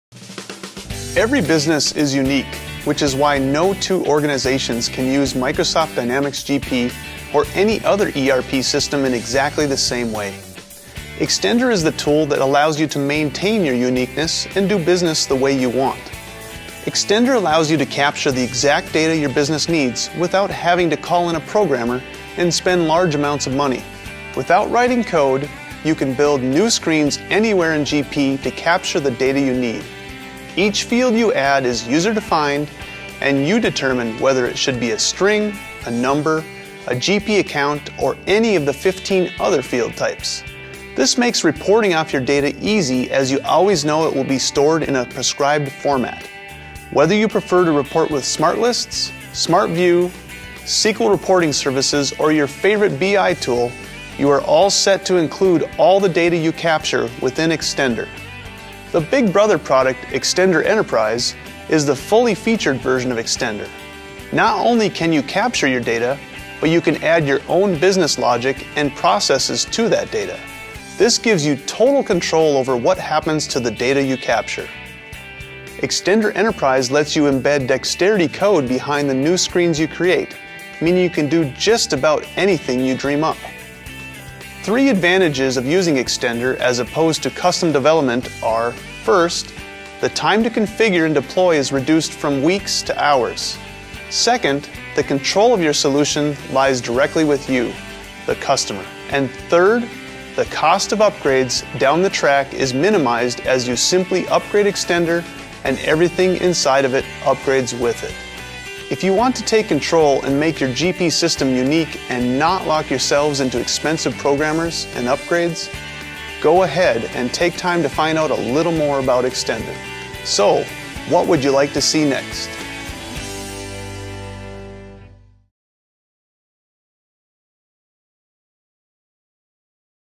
eOne Extender Intro
Every business is unique which is why no two organizations can use Microsoft Dynamics GP or any other ERP system in exactly the same way. Extender is the tool that allows you to maintain your uniqueness and do business the way you want.
Extender allows you to capture the exact data your business needs -- without having to call in a programmer and spend large amounts of money. Without writing code, you can build new screens anywhere in GP to capture the data you need. Each field you add is user defined, and you determine whether it should be a string, a number, a GP account, or any of 15 other field types. This makes reporting off your data easy as you always know it will be stored in a prescribed format. Whether you prefer to report with SmartList, SmartView, SQL reporting Services, or your favorite BI tool you are all set to include all the data you capture within eXtender.

The big brother product, Extender Enterprise, is the fully featured version of Extender. You can not only capture your data, but add your own business logic and processes to that data and then gives you total control over what happens to the data you capture.  Extender Enterprise lets you embed dexterity code behind the new screens you create - meaning you can do just about anything you dream up.